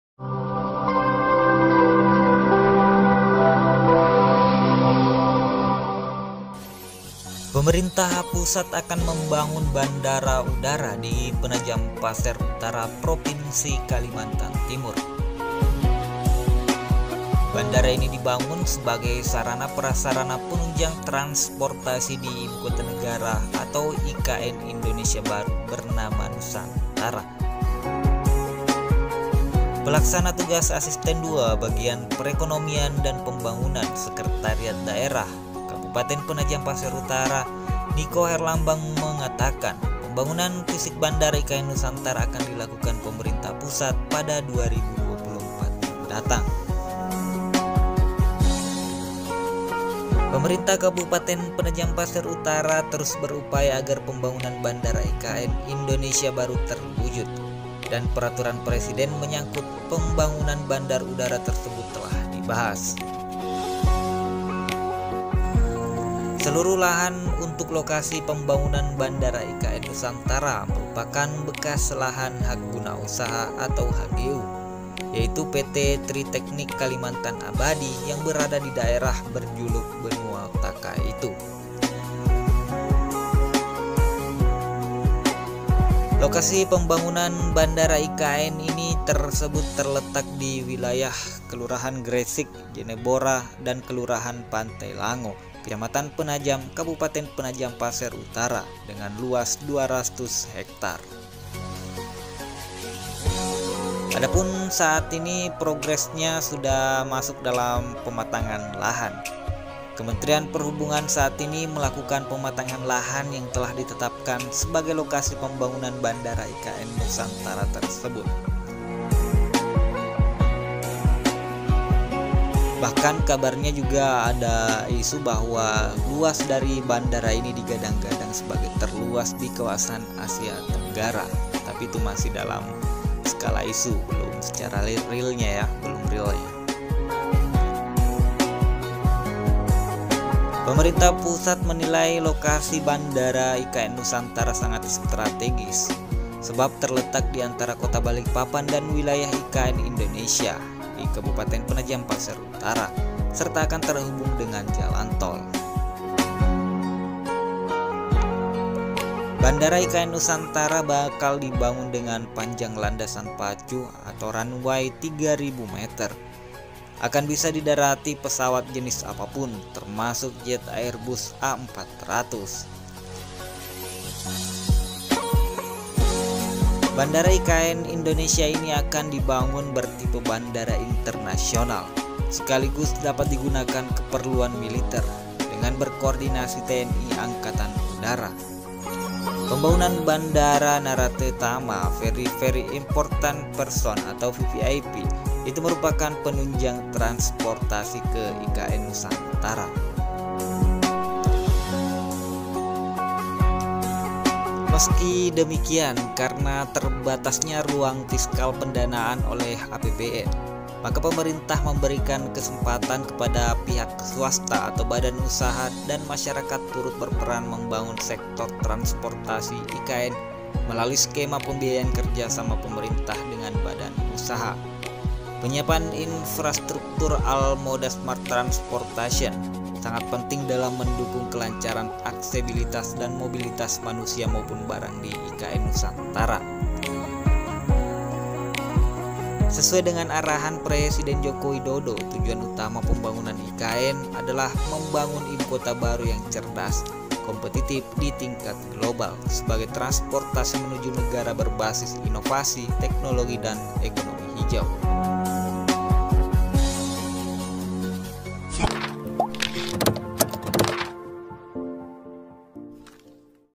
IKN HARI INI‼️Perogres Tekini Pembangunan Bandara di IKN || Terluas Se-Asia Tenggara
Pada video kali ini mimin mengupdate terkait progres pembangunan bandara di wilayah IKN, dimana saat ini sudah memasukti tahap pematangan lahan.

Untuk pembangunan, rencananya dilakukan pada tahun 2024.

Kabarnya juga bahwa pembangunan bandara IKN ini merupakan bandara terluas se Asia Tenggara.